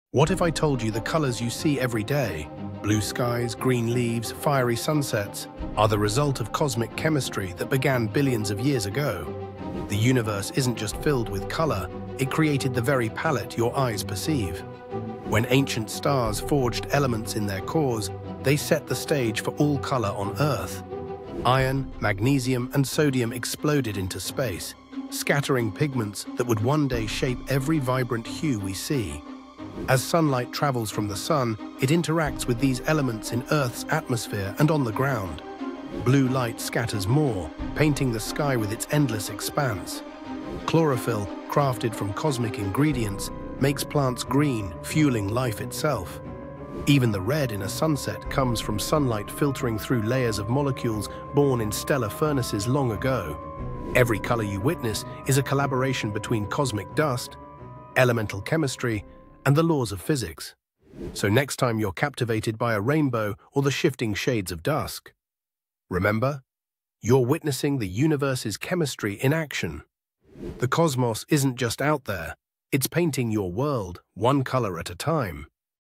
How Cosmic Chemistry Paints Our Universe
Ever wondered where the colors around you come from? The universe is the ultimate artist, and you’re seeing its masterpiece.
also give me some feedback so I can make better video's!